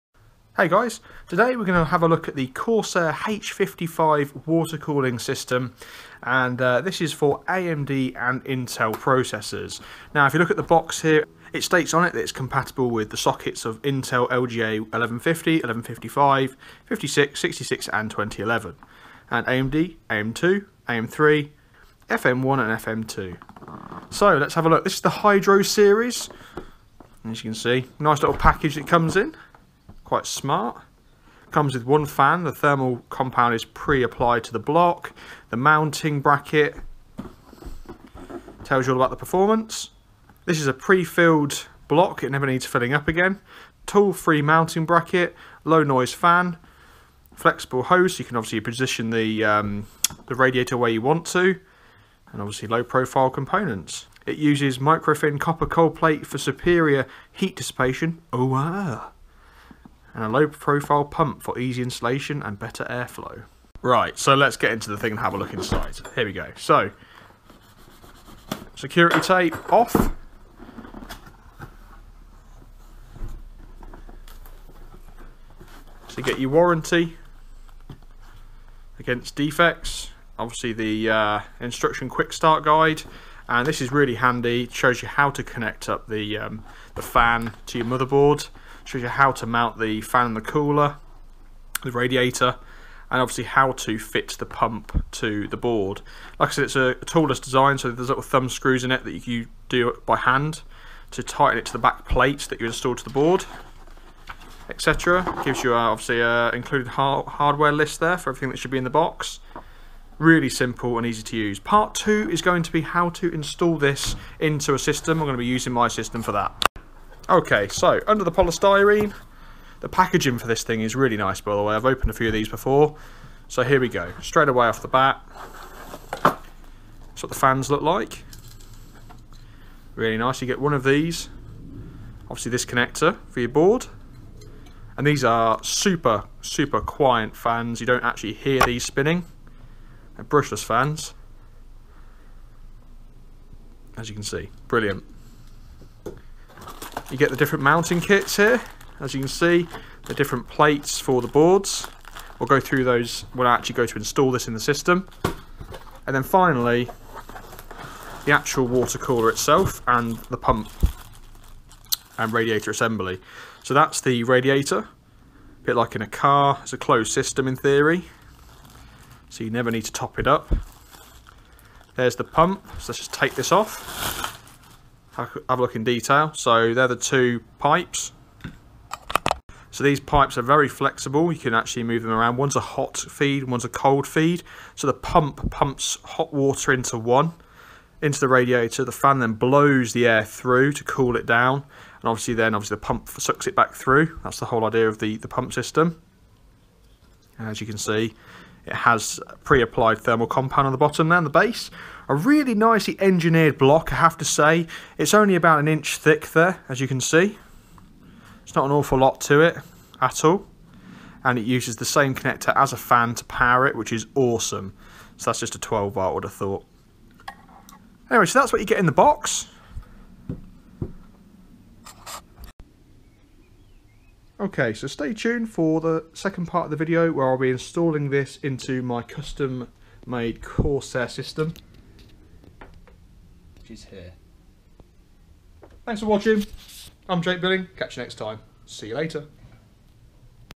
Corsair H55 Hydro Series Watercooler Unboxing
Welcome to this unboxing video for the Corsair H55 Hydro Series Water Cooler. I've had my eyes on one of these for some time now with the release of the new product range I thought it was time I upgraded my current water cooling solution to this.

So far, it look great! Great packaging and easy to follow instructs make this product a no brainer to the enthusiast. The price is also a bonus, it only cost me £39.99!
There will be an installation tutorial video to follow fitting this cooling system to my own gaming desktop.